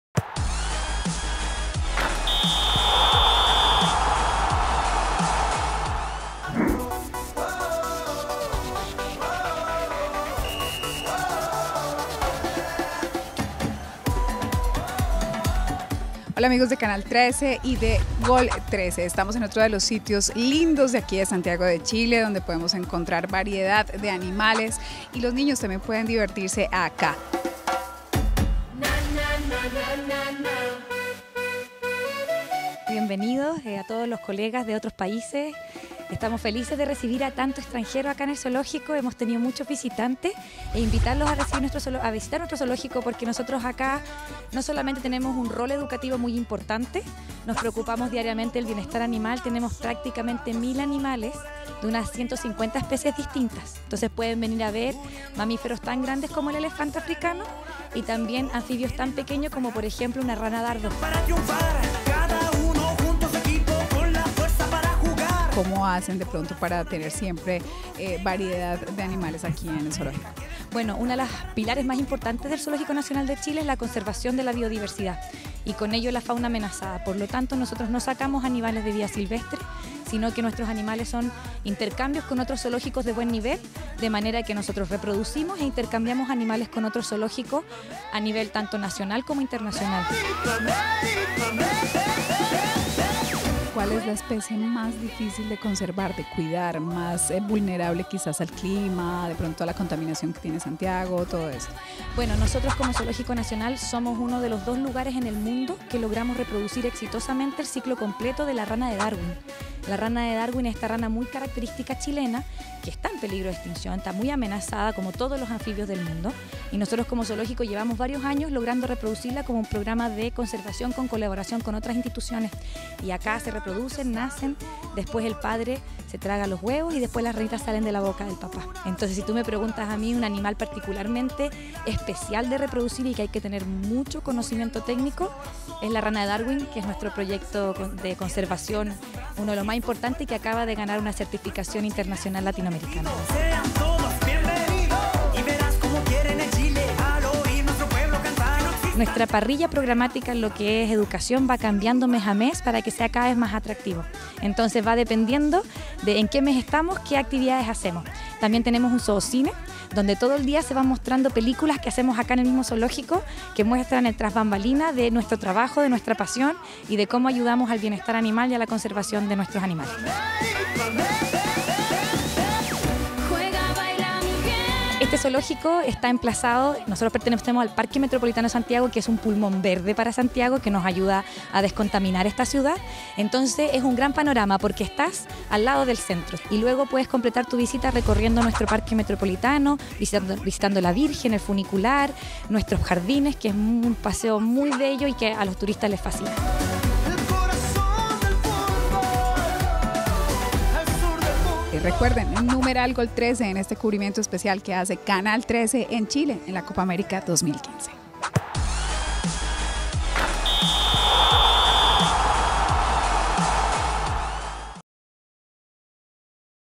Estuvimos con GolTr3ce desde el Zoológico de Santiago
Uno de los atractivos turísticos de la Ciudad de Santiago de Chile es el Zoológico donde existen aproximadamente 150 especies de distintos animales